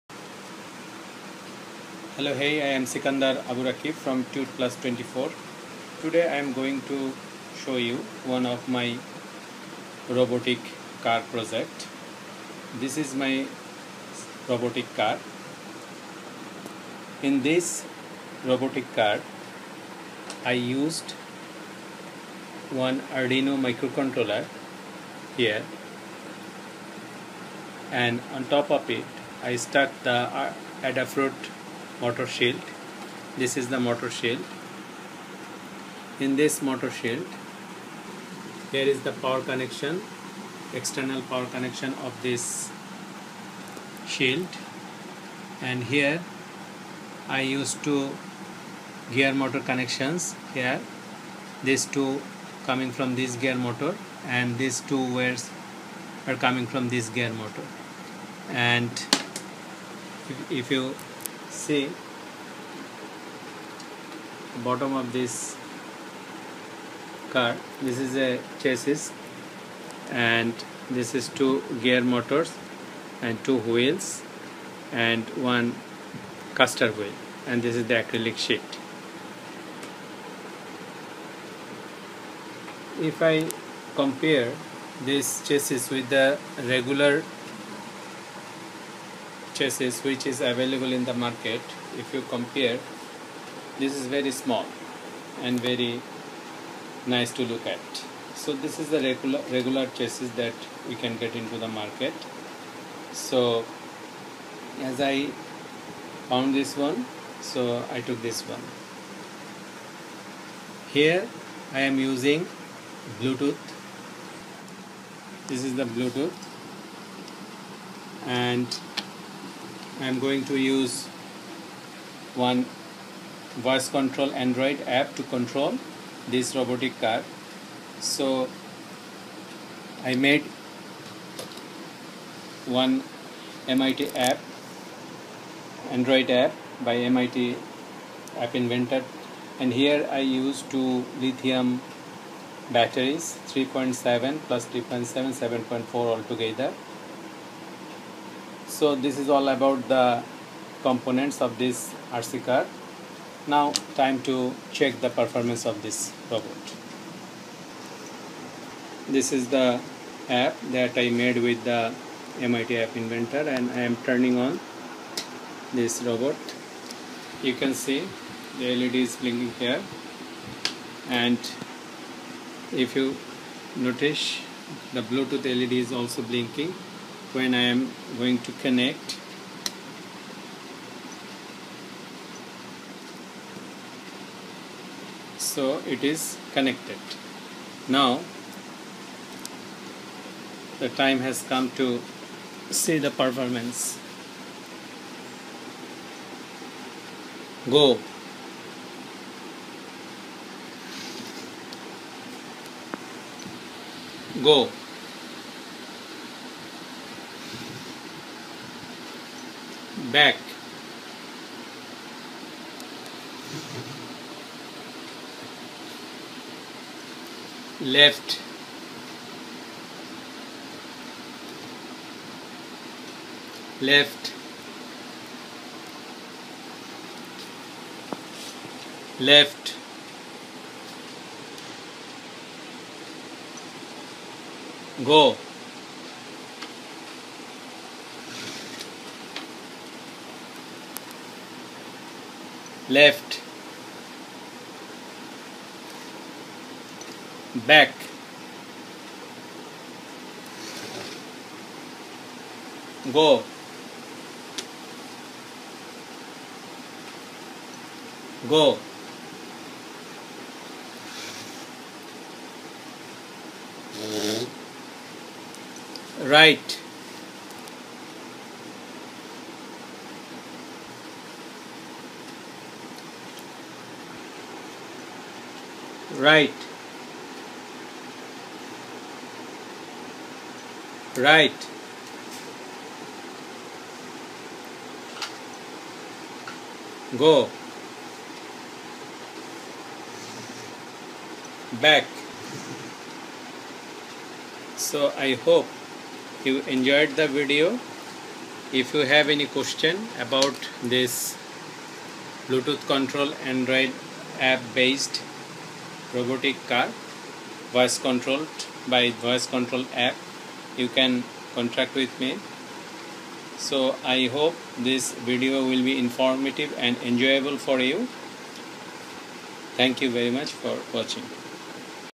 Arduino and Bluetooth based RC car with android voice control app.
In this video I will show you Arduino and Bluetooth based RC car with android voice control app.